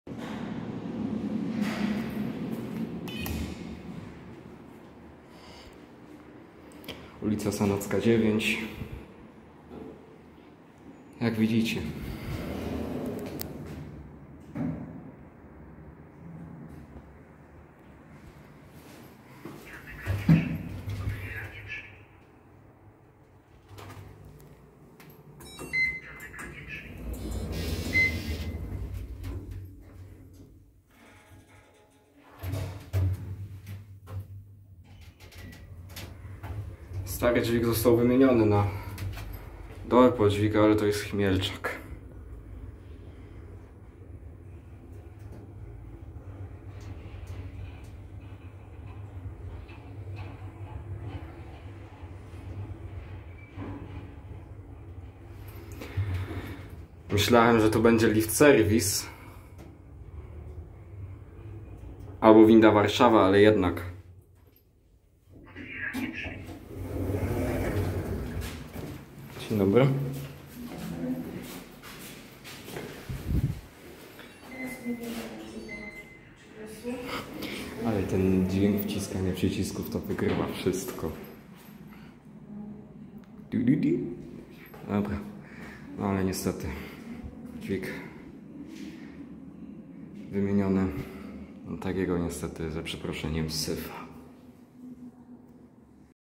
Winda Dolpol-Dźwig z 2020 roku @ Sanocka 9, Wrocław || os. Powstańców Śląskich
Na ostatniej wizycie przy okazji jeszcze sprawdziłem Sanocką 9, gdzie w przetargu wspomniano o wymianie w tej klatce. Tak jak widać w filmie - dźwig wymieniony pod koniec zeszłego roku. Rok temu w wakacjach jeszcze się przejechałem starym dźwigiem w tej klatce. Nawet go nagrałem, ale niestety za dużo ludzi było i nie dało się sam jechać. Jedyne archiwalne nagranie które mam z tego dźwigu powstało w grudniu 2018r i to w wyjątkowo kiepskiej jakości niestety. Był tu ZREMB po mocnej modernizacji przez Techlift, lepiej mówiąc Lewara pod inną nazwą, bo konsola była identyczna. Tak jak w pozostałych klatkach tego bloku - wpakowano do niej drzwi wewnętrzne.
A obecnie w tej klatce spotkamy Dolpol-Dźwiga, a lepiej mówiąc Chmielczaka pod inną nazwą, bo to jest to samo. Jeździ bo jeździ, ale zdecydowanie gorzej niż ta Winda-Warszawa na Studziennej. Tak samo prawdopodobnie jest na Ślicznej 26, Wielkiej 28 i na Zaporoskiej 24.
Jednak dźwięk przycisków w kabinie mnie rozwala.

Dane dźwigu:
Producent: Dolpol-Dźwig
Piętra: 11 (0-10)
Udźwig: 7 osób / 525 kg
Rok produkcji: 2020
Typ windy: Osobowa